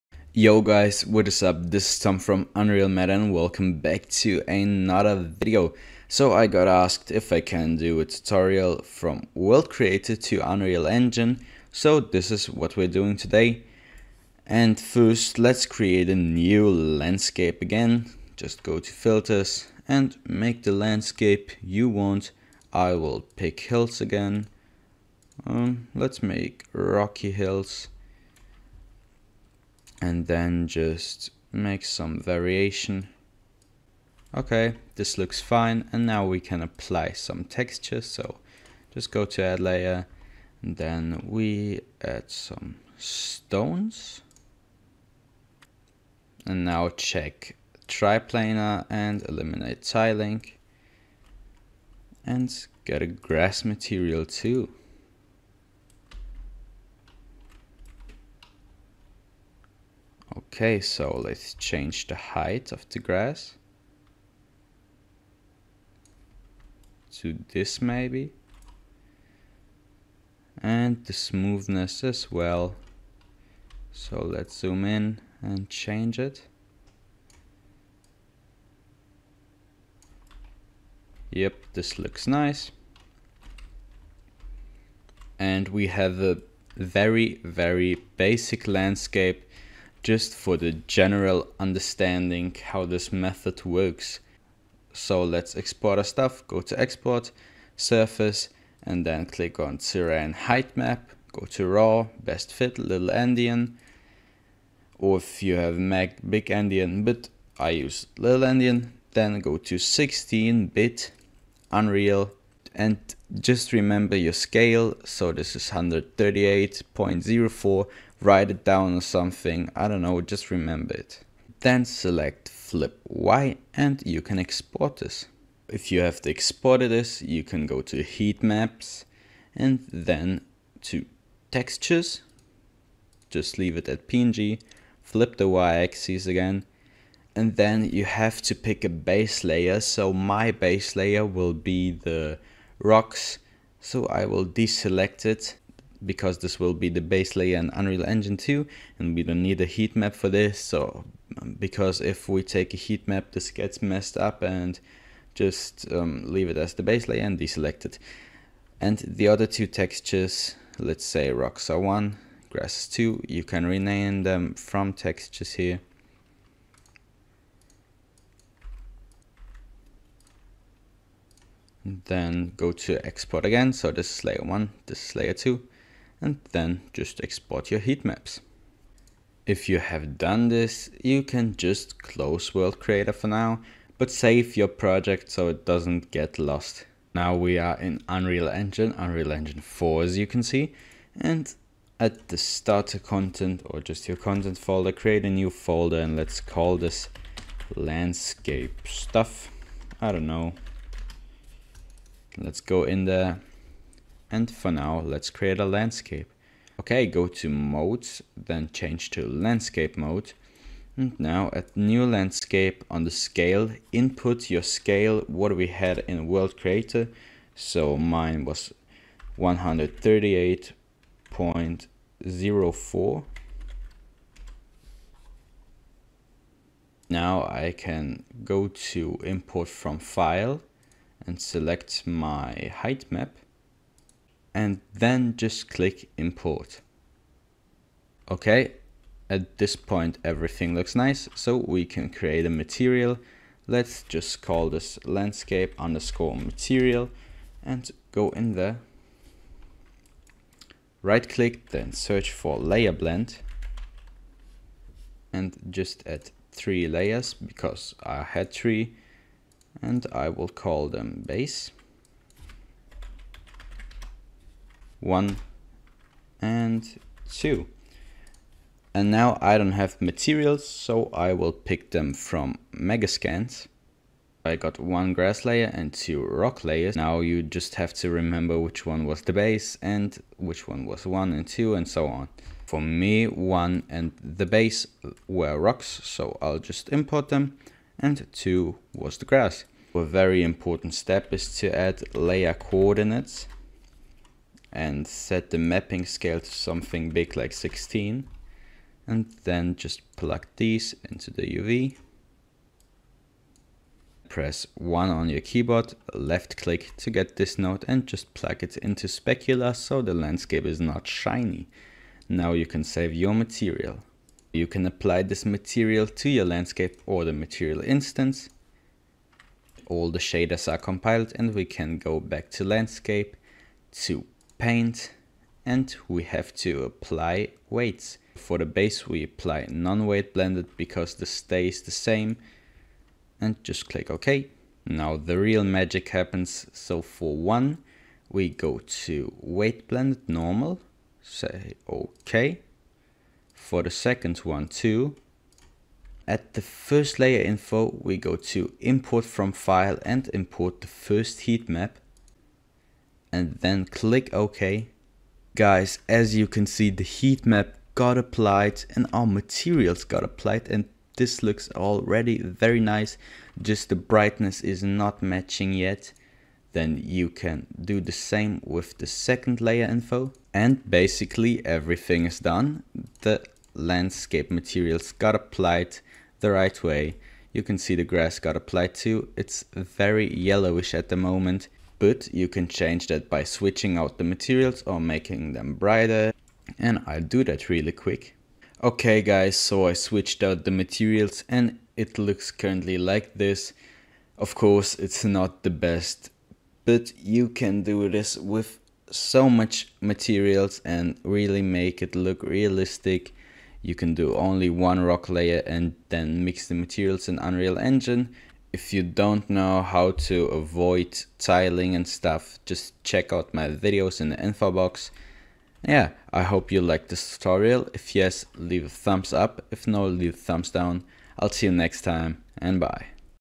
How to export World Creator landscapes to Unreal Engine
Hey guys,
In this tutorial I will show you how export landscapes and heatmaps from World Creator to Unreal Engine 4/5 and then how to texture the landscape. I hope you like the video!